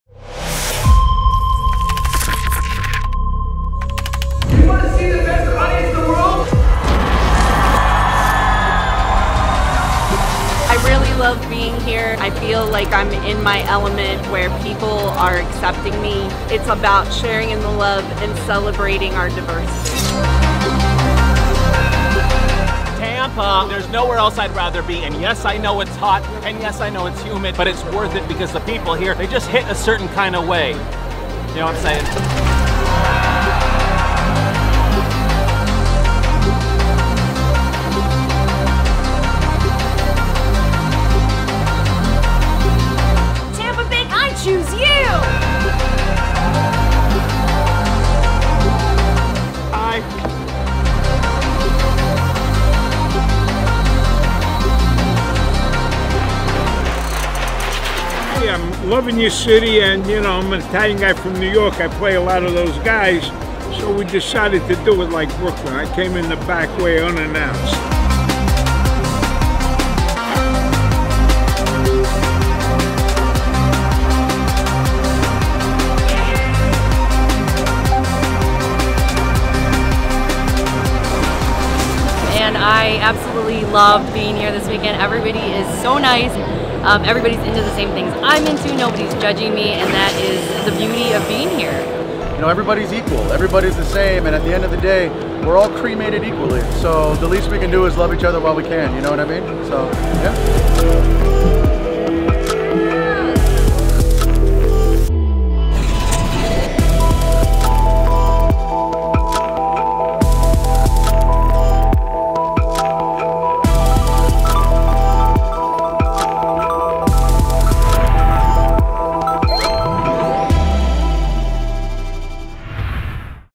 Tampa Bay Comic Convention | Full Highlight
Check out this highlight video of TBCC 2024 including some interviews with celebrities, vendors, con goers, and some of Florida's finest cosplayers.